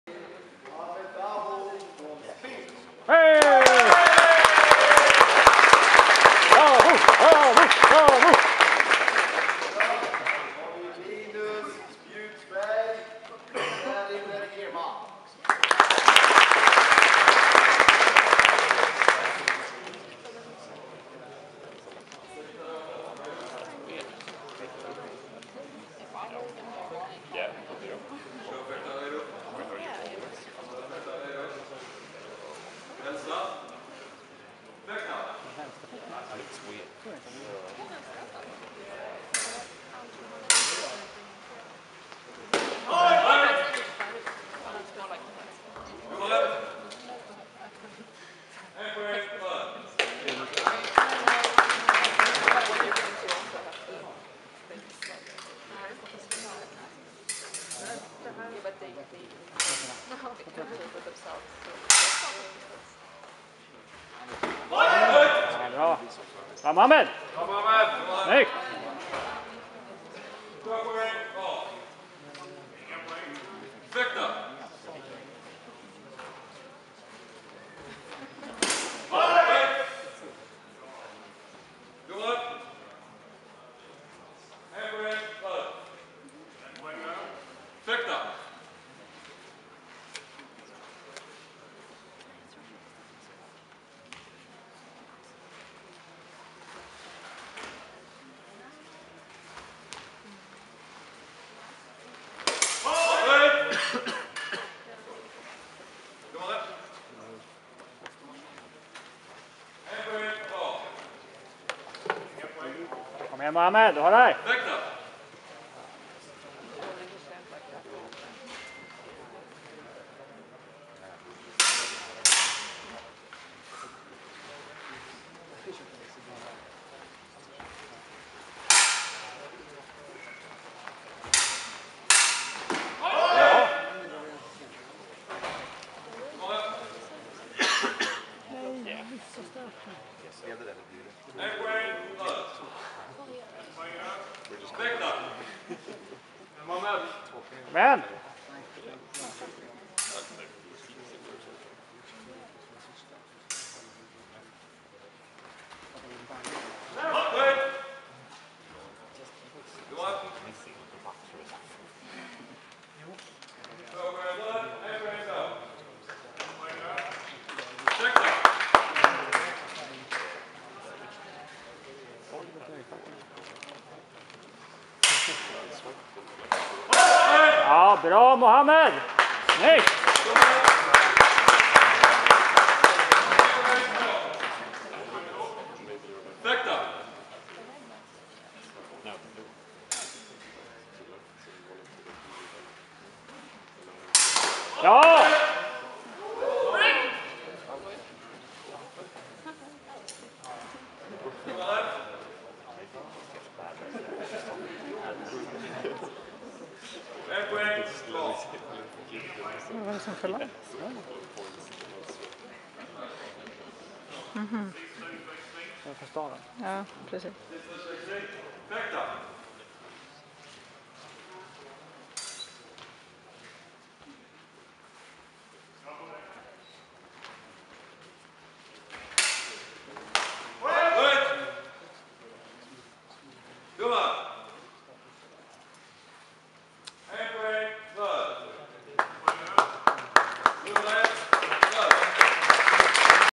SM HEMA 2022, Muhammad Dabo vs Linus Spjutsberg, Sabre, Bronze match (Round 1)
Swedish HEMA championship, taking place in Linköping 2-3 of July 2022.
Sabre, Bronze match, round 1 (best of 3 rounds).
Muhammad Dabo (SPIFF) in blue vs Linus Spjutsberg (Särimner HEMA) in red.

Photography by Malin Engquist.